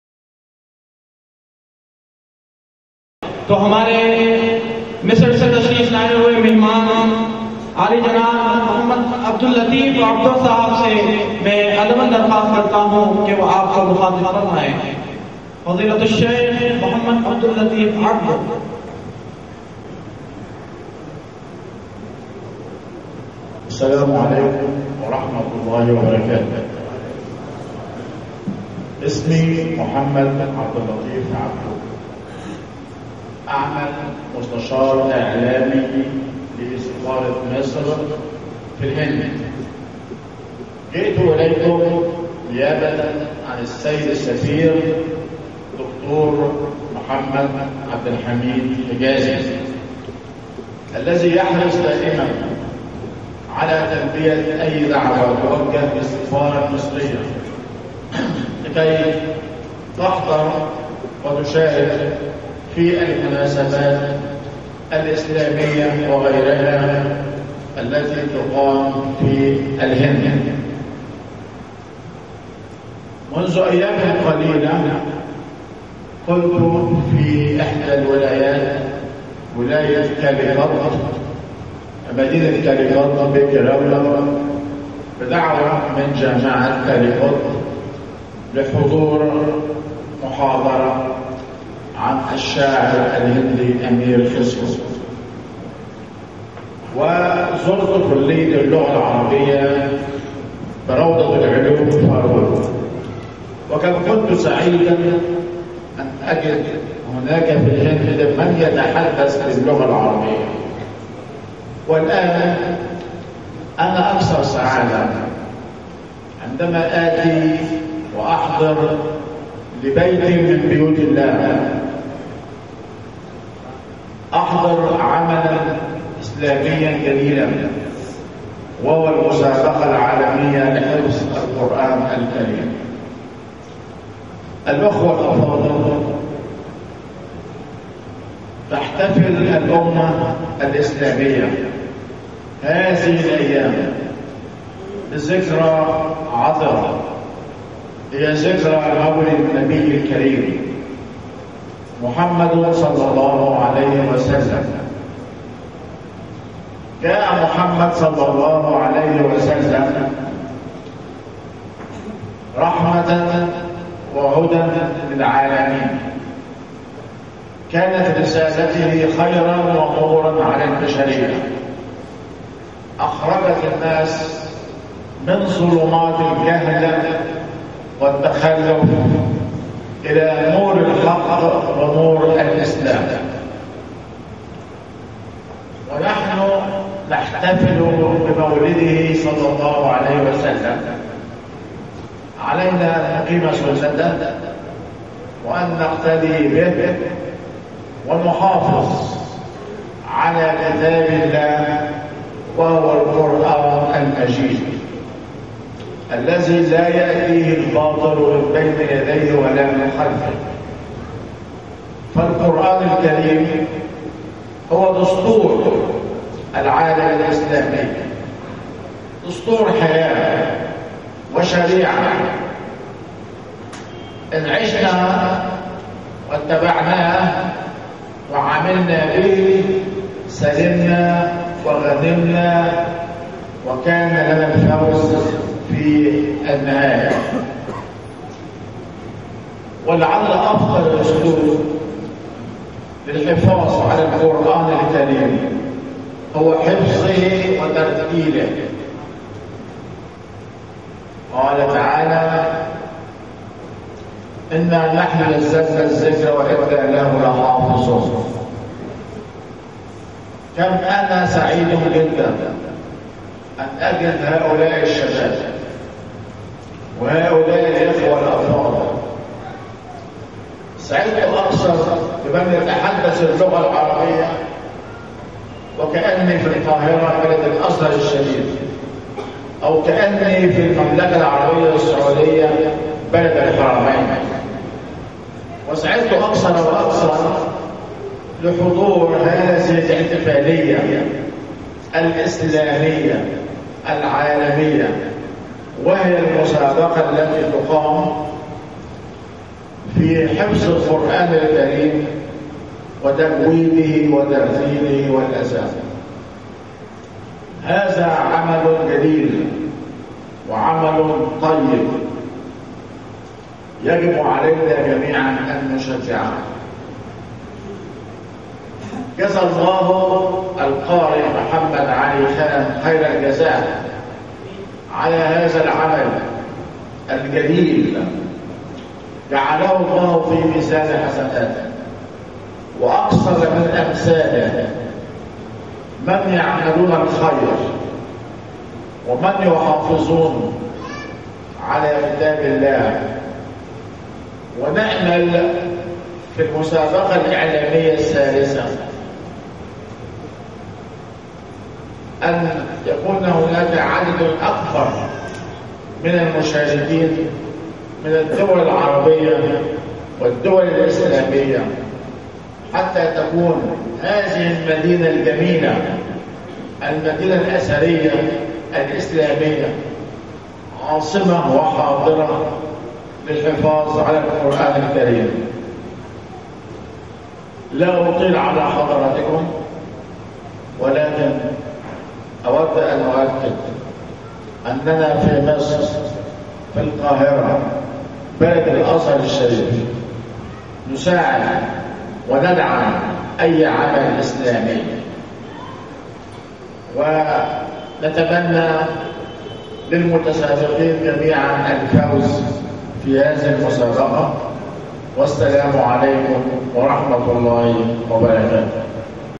Shaik Abdul Lateef Abdo Egypt شیخ عبد الطیف عبدہ حفظ اللہ جمہوریۃ مصر العربیۃ
المسابقۃ العالمیۃ لحفظ القرآن الکریم وتلاوتہ والاذان
تحت اشراف:دار القراءات الباسطیۃ،حیدرآباد الھند

2nd International Competitions in Memorization, Recitation of Holy Qur'aan & Azaan
At: Jama Masjid Basitia, Old Malakpet, Hyderabad, T.S. India
on 26th 27th & 28th February 2010
Under the Management of:
Darul Qirat Al Basitia, Old Malakpet, Hyderabad, T.S. India &
International Holy Quran Competition Committee India
Under the Grand ship of:
Ustazul Qurra Hazrath Maulana Qari Mohammed Ali Khan Sahab
Saudi Awarded
Founder & President Darul Qirat Al Basitia
Imam o Khateeb Jama Masjid Basitia, Hyderbad, India

Convener:
Hafiz o Qari Moulvi Mohammed Yunus Ali Khan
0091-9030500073

دوسرا عالمی مسابقہ حفظ، قراءت قرآن کریم و اذان
بتاریخ 26 تا 28 فروری 2010
بمقام جامع مسجد باسطیہ، قدیم ملک پیٹ، حیدرآباد، تلنگانہ، انڈیا
زیر اہتمام دار القراءت الباسطیہ، قدیم ملک پیٹ، حیدرآباد، انڈیا
زیر سرپرستی و زیر نگرانی
استاذ الاساتذہ استاذ القراء حضرت مولانا قاری محمد علی خان صاحب دامت برکاتہم
سعودی انعام یافتہ
بانی و صدر دار القراءت الباسطیہ
امام و خطیب جامع مسجد باسطیہ
ڈائرکٹر عالمی مسابقہ قرآن کریم کمیٹی انڈیا

کنوینئر عالمی مسابقہ
حافظ و قاری مولوی محمد یونس علی خان
مبائل نمبر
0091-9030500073